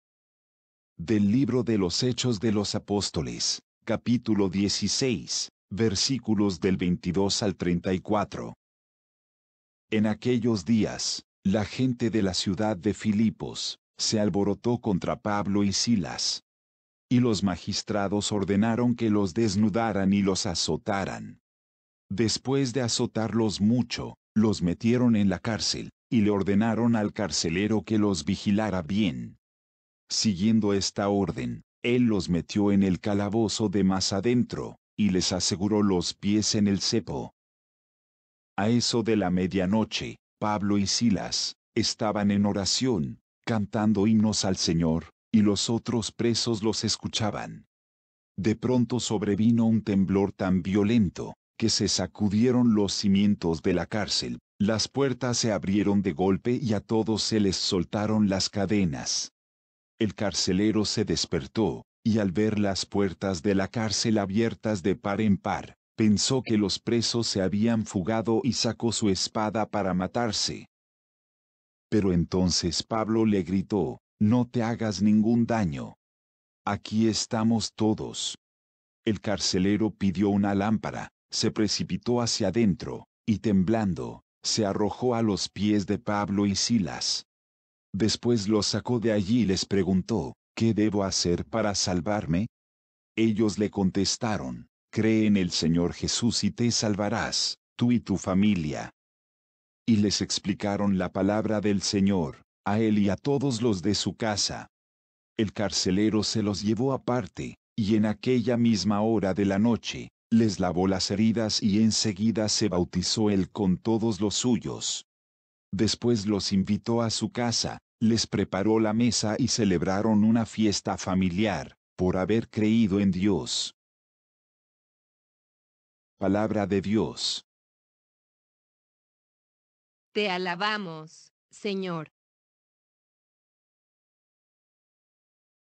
HECHOS DE LOS APÓSTOLES 16 22 34
Del libro de los Hechos de los Apóstoles: 16, 22-34

En aquellos días, la gente de la ciudad de Filipos se alborotó contra Pablo y Silas, y los magistrados ordenaron que los desnudaran y los azotaran. Después de azotarlos mucho, los metieron en la cárcel y le ordenaron al carcelero que los vigilara bien. Siguiendo esta orden, él los metió en el calabozo de más adentro y les aseguró los pies en el cepo.

A eso de la medianoche, Pablo y Silas estaban en oración, cantando himnos al Señor, y los otros presos los escuchaban. De pronto sobrevino un temblor tan violento, que se sacudieron los cimientos de la cárcel, las puertas se abrieron de golpe y a todos se les soltaron las cadenas. El carcelero se despertó, y al ver las puertas de la cárcel abiertas de par en par, pensó que los presos se habían fugado y sacó su espada para matarse. Pero entonces Pablo le gritó: "No te hagas ningún daño; aquí estamos todos". El carcelero pidió una lámpara, se precipitó hacia dentro, y temblando, se arrojó a los pies de Pablo y Silas. Después los sacó de allí y les preguntó: "¿Qué debo hacer para salvarme?" Ellos le contestaron: "Cree en el Señor Jesús y te salvarás, tú y tu familia". Y les explicaron la Palabra del Señor a él y a todos los de su casa. El carcelero se los llevó aparte, y en aquella misma hora de la noche les lavó las heridas y en seguida se bautizó él con todos los suyos. Después los invitó a su casa, les preparó la mesa y celebraron una fiesta familiar por haber creído en Dios.

Te alabamos, Señor.